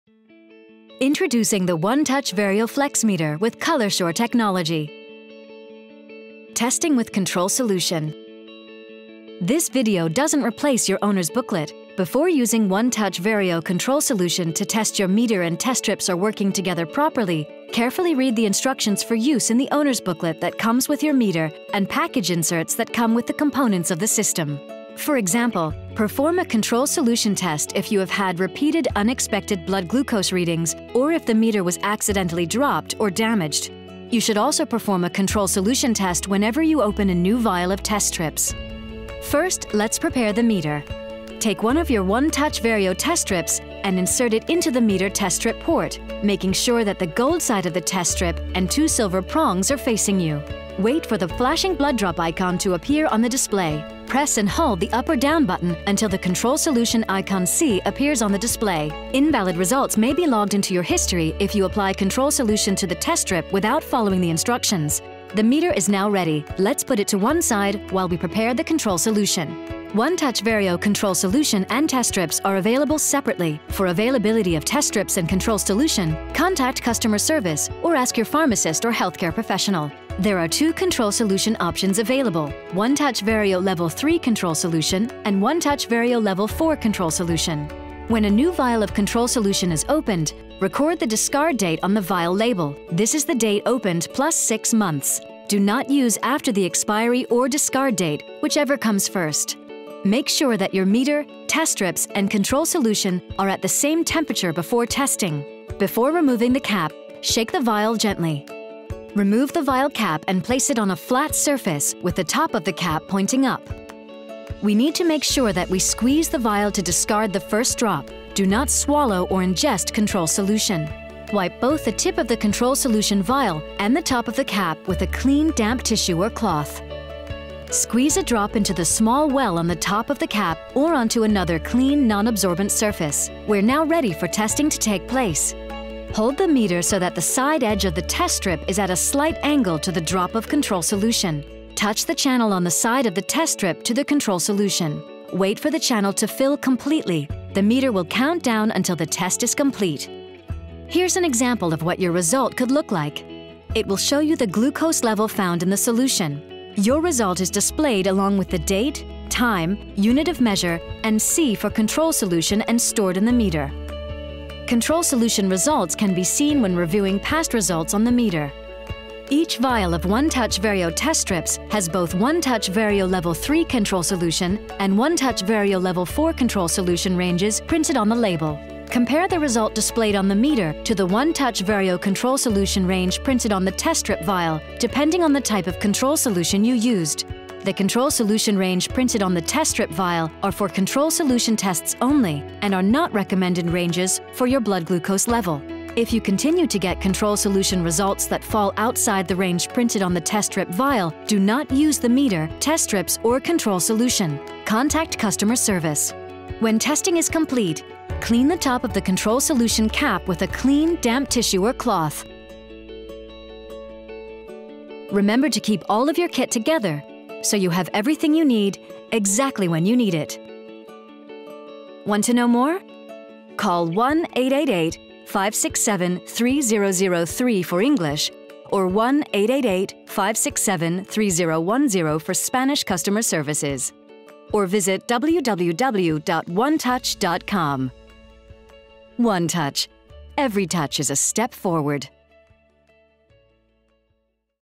OneTouch Verio Flex® meter: How to do a control solution test
Learn how to perform a control solution test with your OneTouch Verio Flex® blood glucose meter to check the meter is working correctly.  For example, if you have had repeated unexpected blood glucose readings or if the meter was accidently dropped or damaged.  You should also perform a control solution test each time you open a new vial of OneTouch Verio® test strips.
Before using the OneTouch Verio Flex® meter to test your blood glucose, carefully read the Owner’s Booklet and inserts that come with the components of the system.